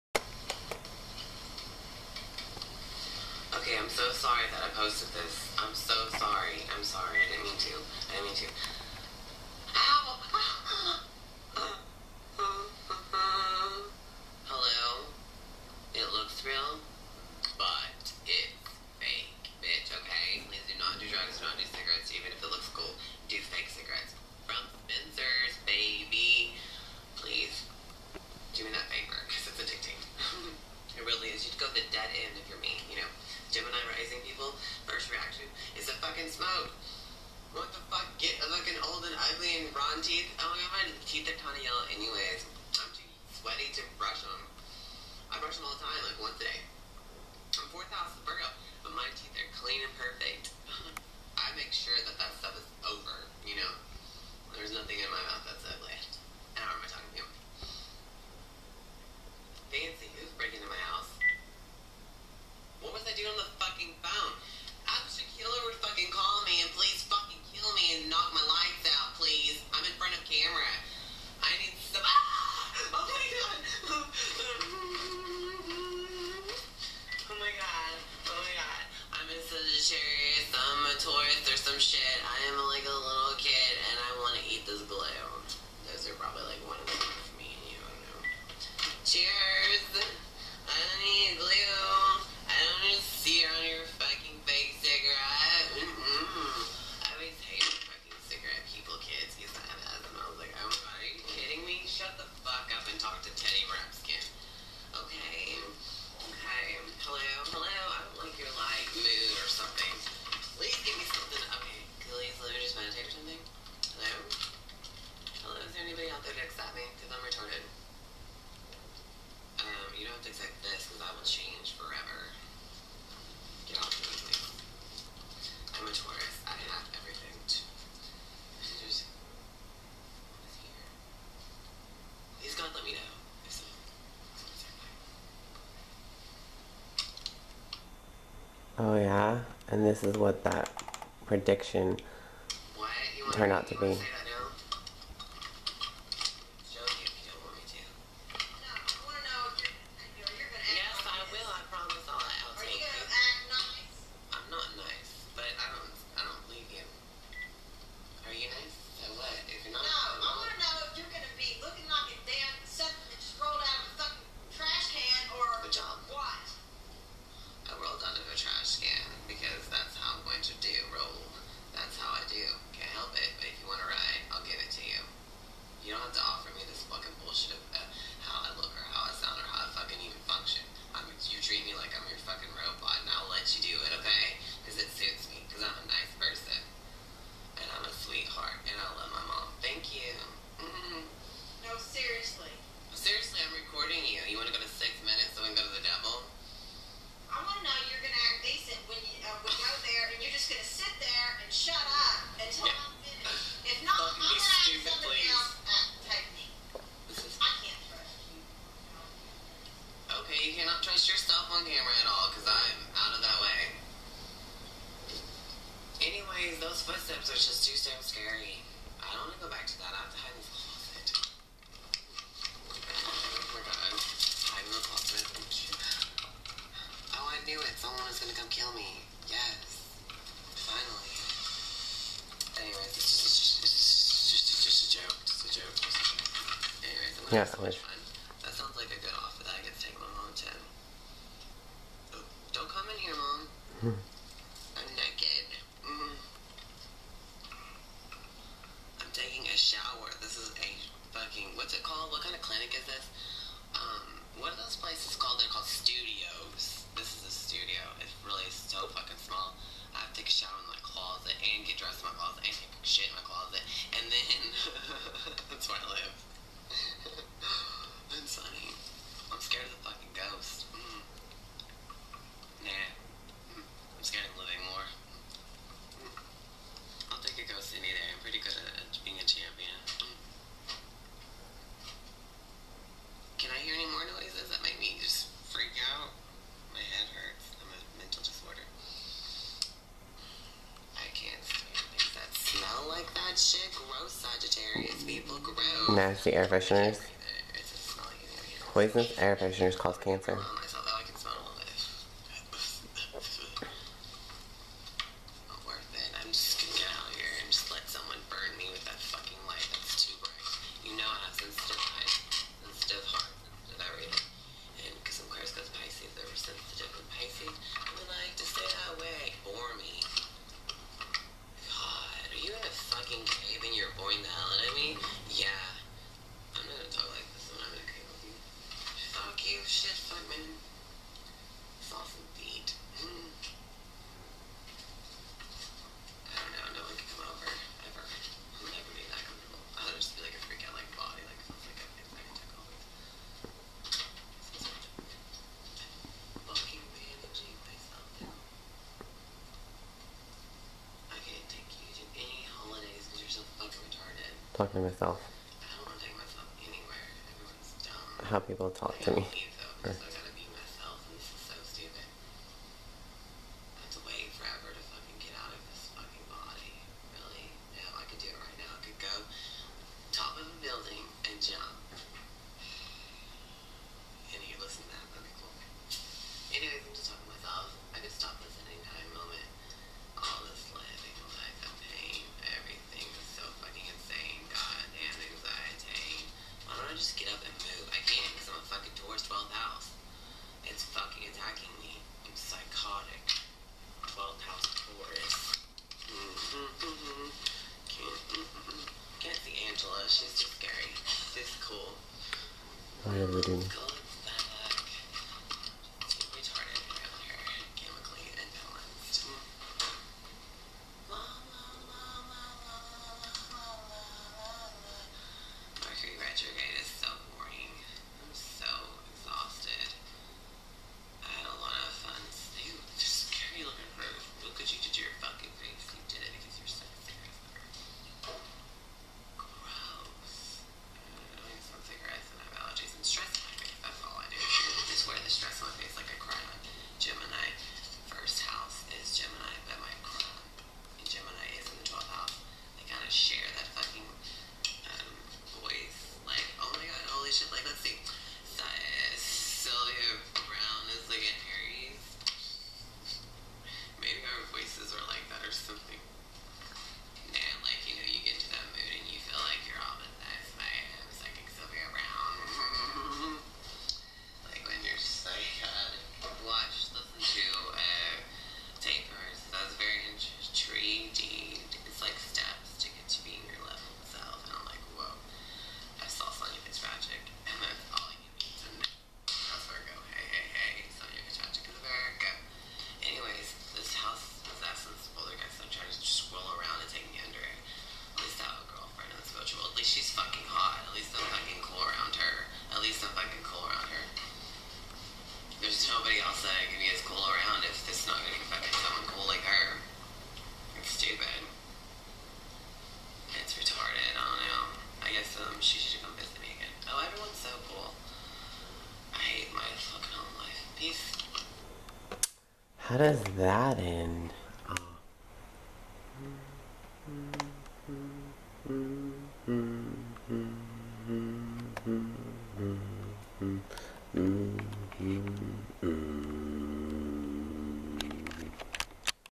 4️⃣th 🏠House ♍Virgo Mommy 🦌Dearest exsample
LIbra riseing revealed duh b.s. the 4th house Virgo nonsense  duh
This reminds me of Cow & Chicken*s Parents LoL
Saturn 5th house Parent duh.*